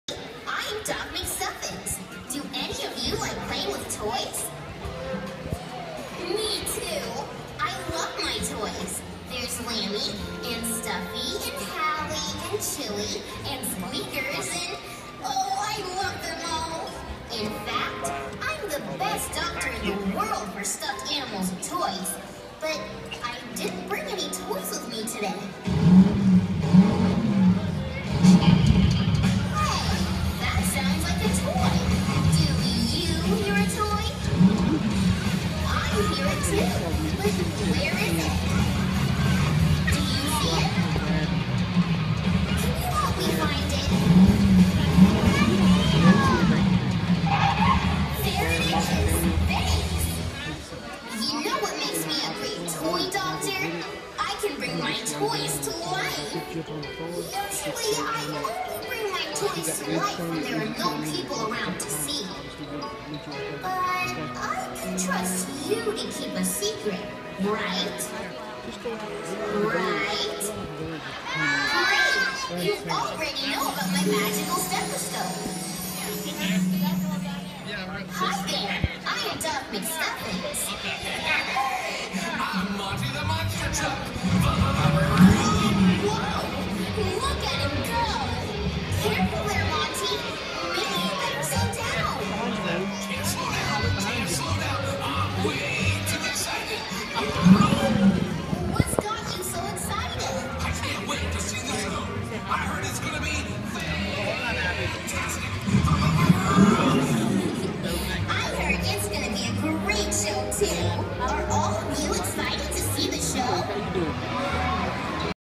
Disney Junior Live: Pirate and Princesses Tour!
Princess Sophia and Jake and the Neverland Pirates adventures.